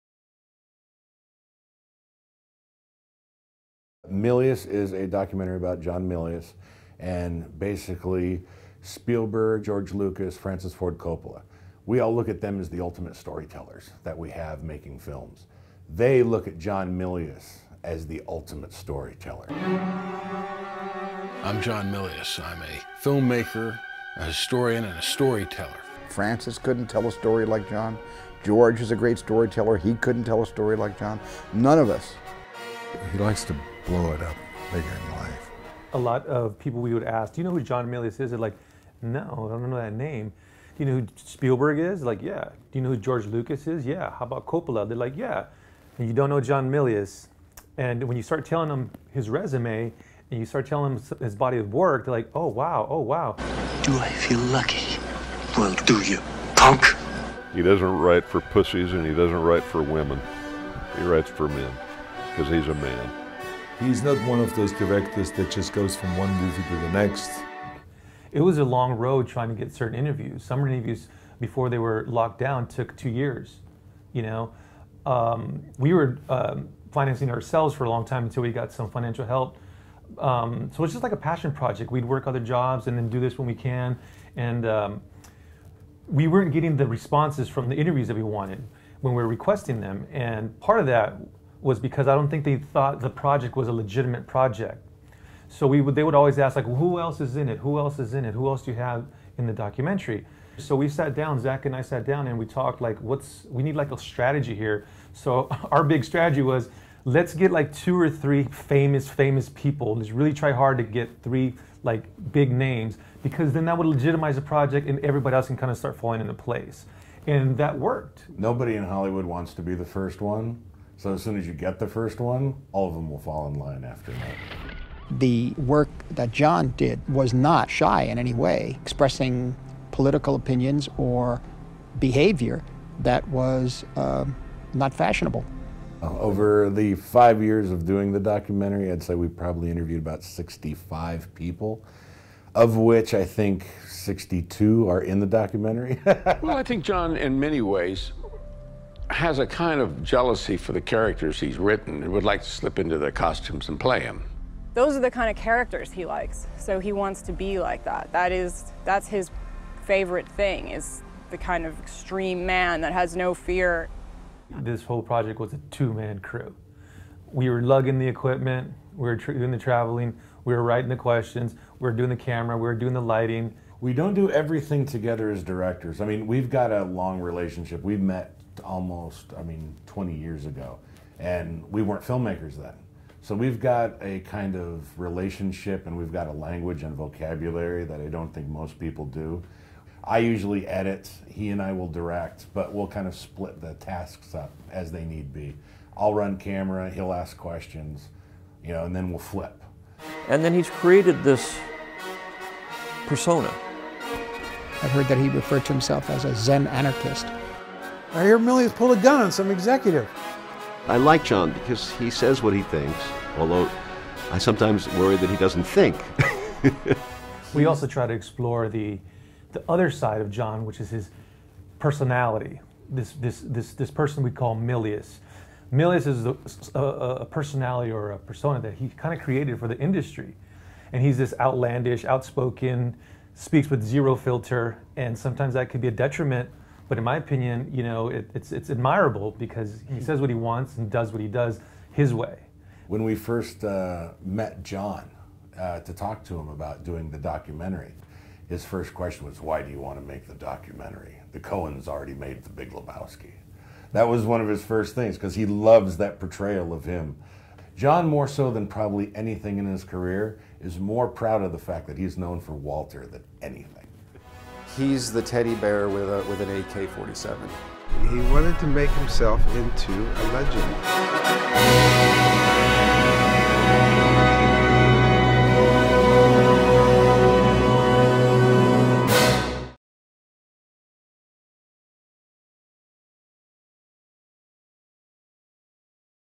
MILIUS - SOHK.TV interview with directors Zak Knutson and Joey Figueroa
If John Milius had written only the line 'I love the smell of napalm in the morning' or contributed to Quint's USS Indianapolis monologue in Jaws, he would have earned his place in film history. He did these things, of course, and more. While his script for Apocalypse Now is the most celebrated work, the films he directed in the 1970s, such as Dillinger and Big Wednesday, were as brilliant as many made in the golden age when Milius and his fellow film school graduates were staking their claim to Hollywood.

In this bold documentary Milius is the Movie Brats' unifying figure, with Lucas, Spielberg, Scorsese and Coppola among the talking heads lining up to pay tribute. If his peers acknowledge his worth, few in Hollywood system could put up with his gun-toting, right-wing politics and macho antics; despite box-office success with Conan the Barbarian and Red Dawn, for the studios Milius has long been persona non grata. His TV series Rome blazed a comeback trail but things are yet to go to plan; a recurring theme in the life of an influential, fascinating filmmaker and a true maverick.

SOHK.TV interview the two directors of MILIUS, Zak Knutson and Joey Figueroa.
With thanks to the BFI London Film Festival.